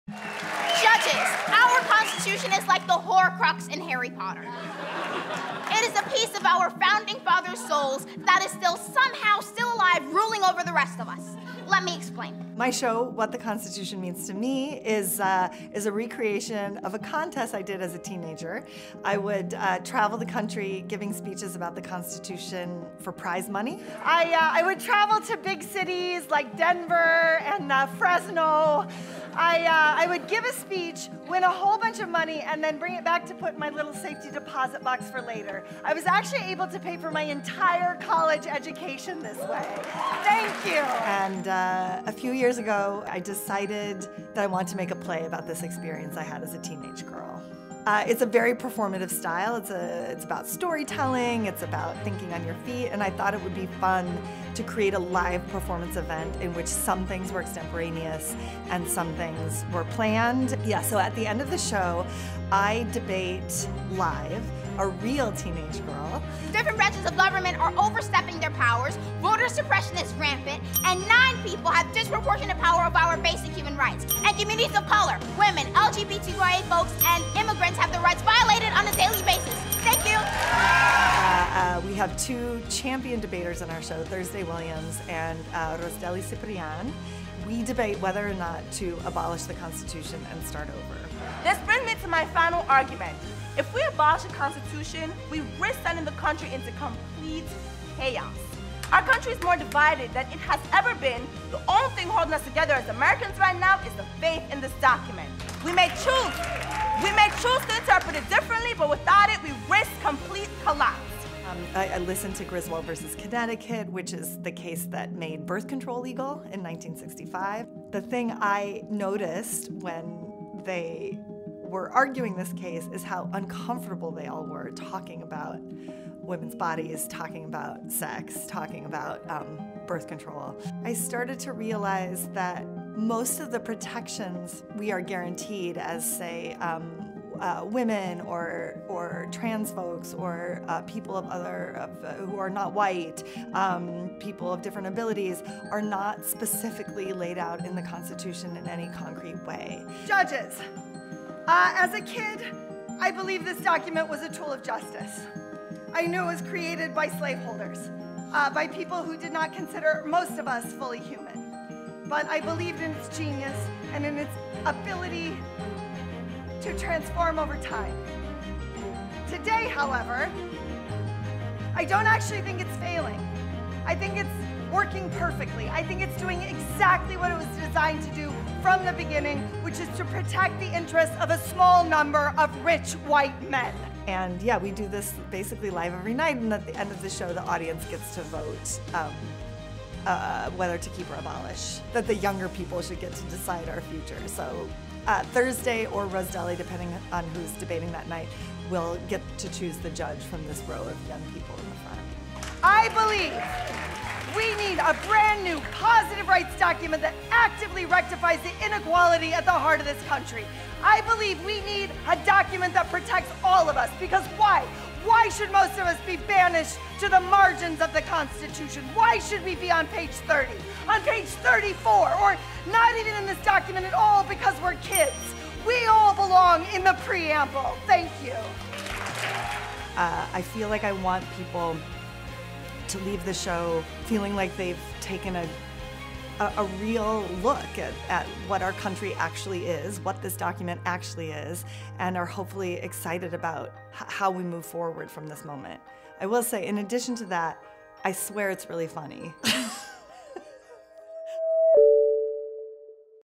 Playwright and Star of 'What the Constitution Means to Me' on Abolishing the Constitution
In the autobiographical Broadway play 'What the Constitution Means to Me' writer Heidi Schreck examines if the document is relevant today -- and how it has affected centuries of women.